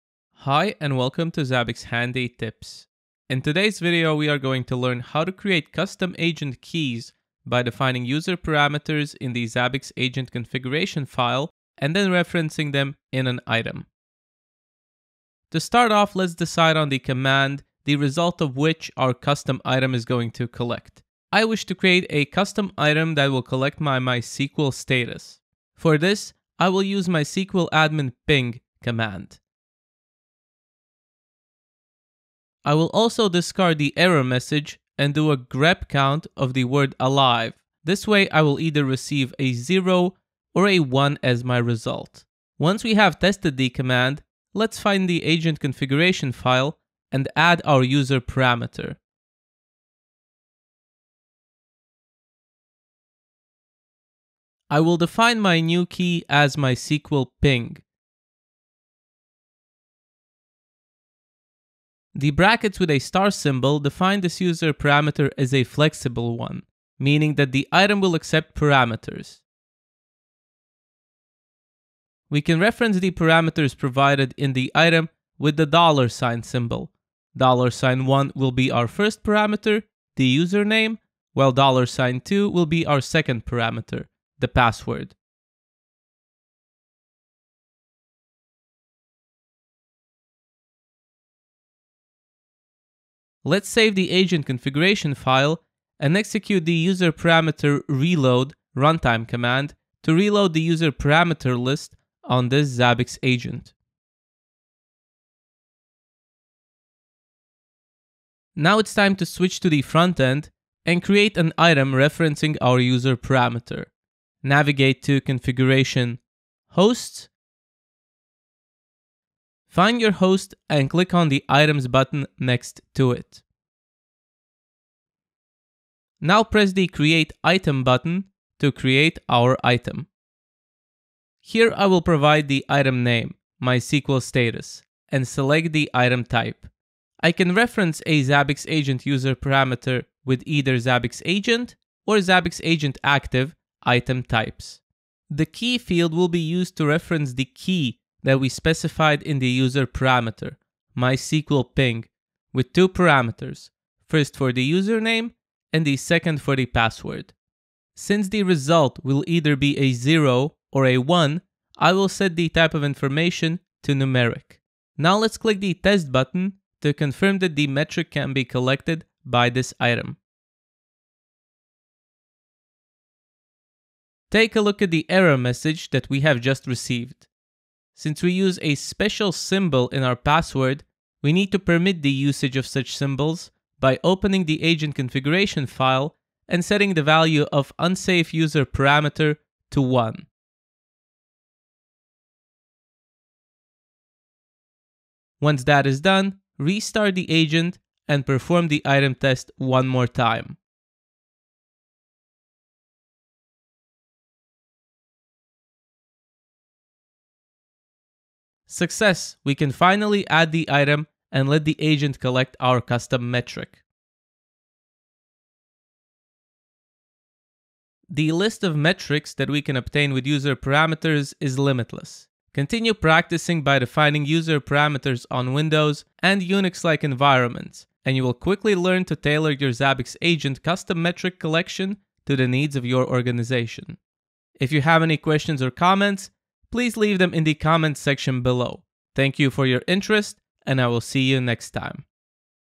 Zabbix Handy Tips: Collecting custom metrics with Zabbix agent user parameters
Zabbix Handy Tips - is byte-sized news for busy techies, focused on one particular topic. In this video, we will learn how to extend the functionality of Zabbix agent and create new custom item keys by defining Zabbix agent user parameters.

to byte-sized news "Zabbix Handy tips" for busy techies! No spam, ever.